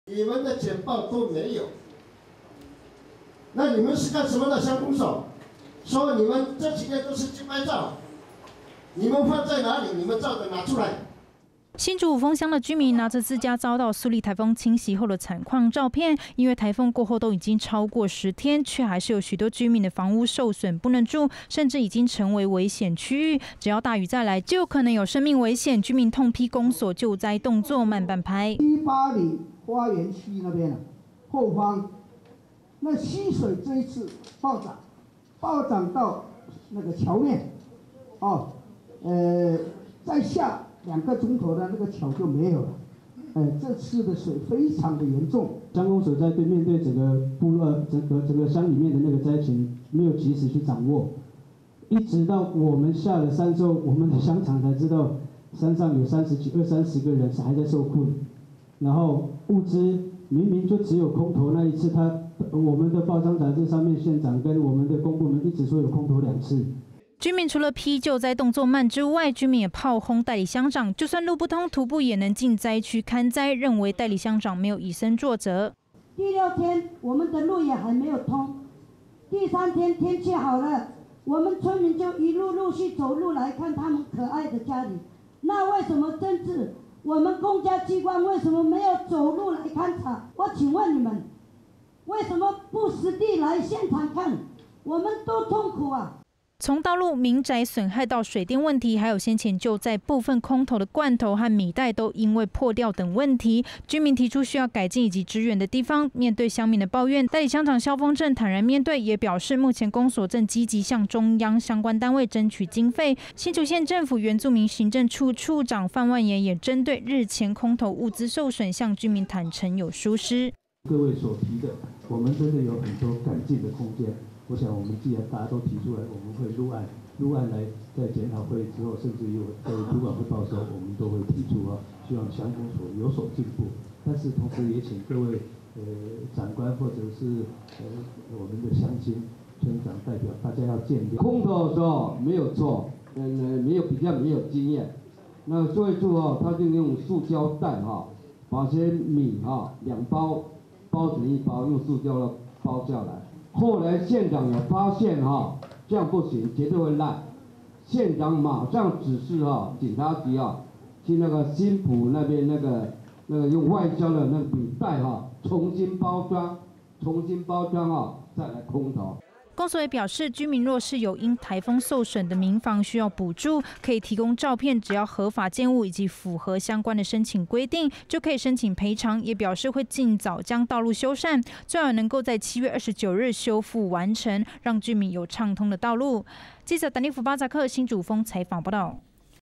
五峰救災慢半拍 民怨代理鄉長失職 20130725
【Tanivu/bazak】

新竹五峰鄉的居民拿著自家遭到蘇力颱風侵襲後的慘況，因為颱風過後都已經超過十天，卻還是有許多居民的房屋受損不能住，甚至已經成為危險區域，只要大雨再來，就可能會有生命危險，居民痛批公所救災動作慢半拍。

除了批救災動作慢之外，居民也炮轟代理鄉長，就算路不通，徒步也能進災區勘災，認為代理鄉長沒有以身作則。從道路、民宅損害到水電問題，還有先前救災部分空投的罐頭和米袋因為破掉等問題，居民紛紛提出需要改進以及支援的地方。

面對鄉民的抱怨，代理鄉長蕭烽政坦然面對，也表示目前公所也正積極向中央相關單位爭取經費，新竹縣政府原住民族行政處長范萬炎，也針對日前空投物資損毀向居民坦承有疏失。

公所也表示，居民若是有因颱風受損的民房需要補助，可以提供照片，只要合法建物以及符合相關申請規定，就可以申請賠償，也表示會盡早將道路修繕，最晚能夠在7月29日修復完成，讓居民有暢通的道路。